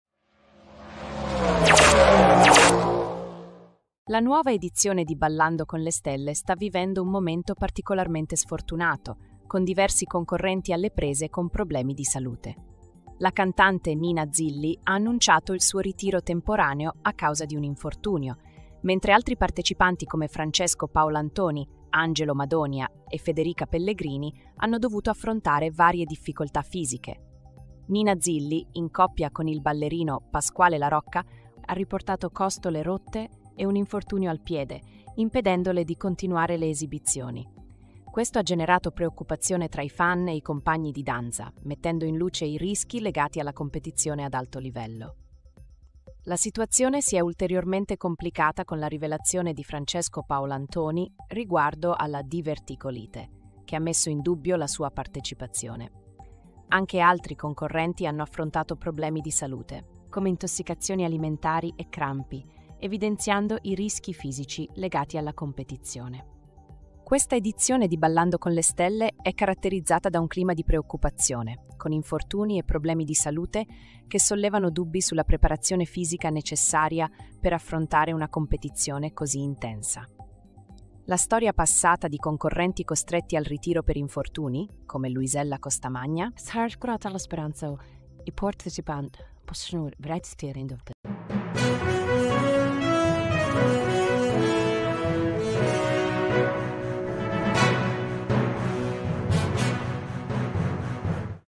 BALLANDO CON LE STELLE: CONCORRENTI IN DIFFICOLTÀ, RISCHI PER LA SALUTE
La nuova edizione di Ballando con le stelle sta vivendo un momento particolarmente sfortunato, con diversi concorrenti alle prese con problemi di salute. La cantante Nina Zilli ha annunciato il suo ritiro temporaneo a causa di un infortunio, mentre altri partecipanti come Francesco Paolantoni, Angelo Madonia e Federica Pellegrini hanno dovuto affrontare varie difficoltà fisiche.

Nina Zilli, in coppia con il ballerino Pasquale La Rocca, ha riportato costole rotte e un infortunio al piede, impedendole di continuare le esibizioni. Questo ha generato preoccupazione tra i fan e i compagni di danza, mettendo in luce i rischi legati alla competizione ad alto livello.

La situazione si è ulteriormente complicata con la rivelazione di Francesco Paolantoni riguardo alla diverticolite, che ha messo in dubbio la sua partecipazione. Anche altri concorrenti hanno affrontato problemi di salute, come intossicazioni alimentari e crampi, evidenziando i rischi fisici legati alla competizione.

Questa edizione di Ballando con le stelle è caratterizzata da un clima di preoccupazione, con infortuni e problemi di salute che sollevano dubbi sulla preparazione fisica necessaria per affrontare una competizione così intensa. La storia passata di concorrenti costretti al ritiro per infortuni, come Luisella Costamagna, fa sorgere la speranza che i partecipanti attuali possano recuperare rapidamente e tornare in pista.